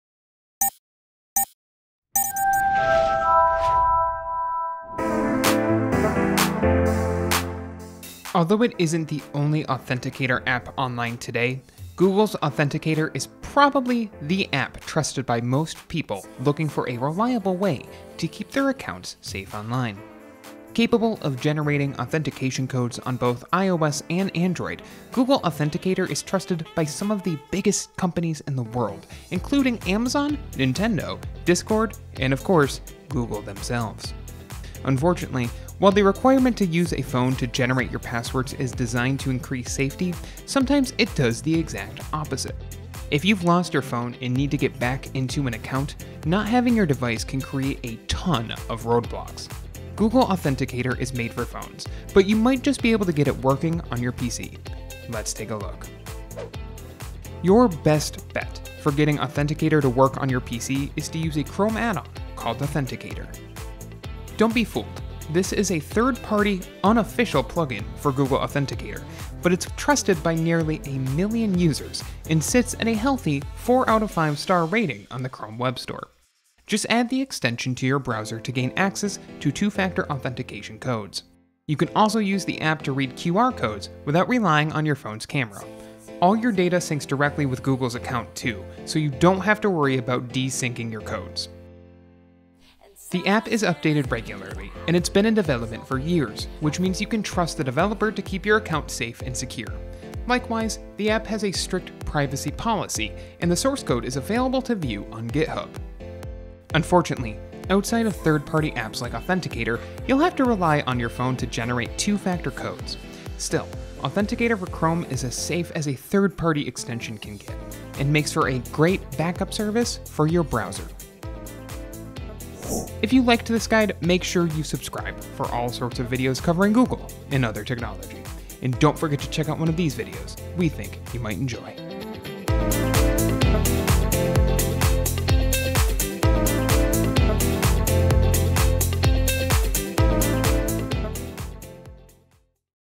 How to Use Google Authenticator with a PC!
If you’ve lost your phone and need to get back into an account, not having your device can create a ton of roadblocks. Google Authenticator is made for phones, but you might just be able to get it working on your PC. Let’s take a look.